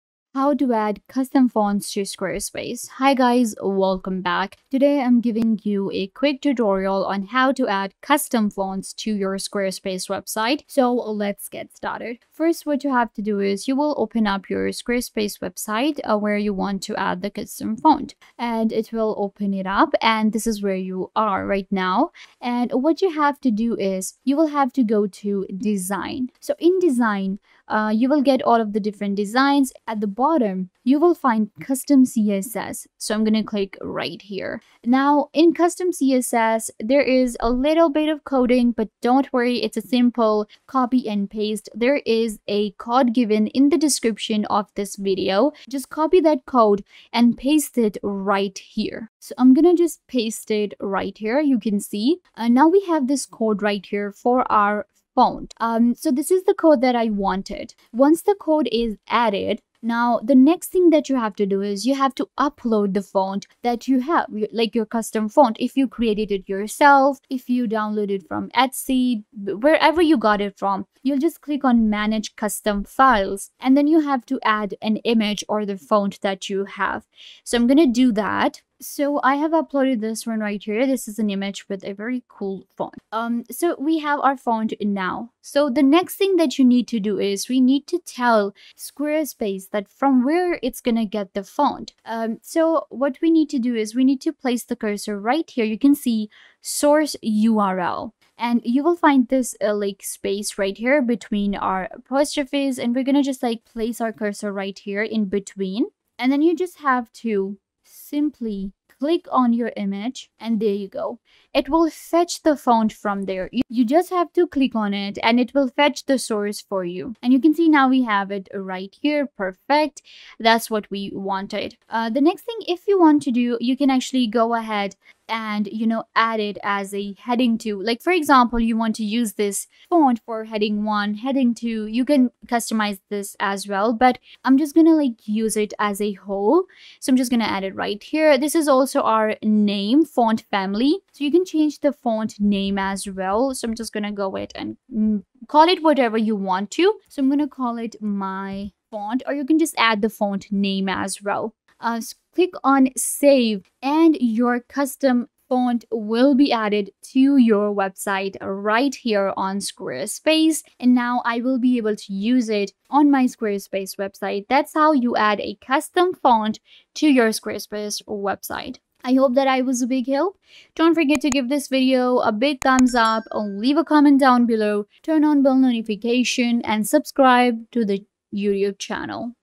How To Add Custom Fonts To Squarespace 2026 (Easy)
In this video I show you How To Add Custom Fonts To Squarespace. It is really easy to do and learn to do it in just a few minutes by following this super helpful tutorial to get a better understanding of everything.

Copy the code below:
     font-family: 'Quentin';
     src: url (' ');
     font-weight: regular;

h2 {
     font-family: 'Quentin';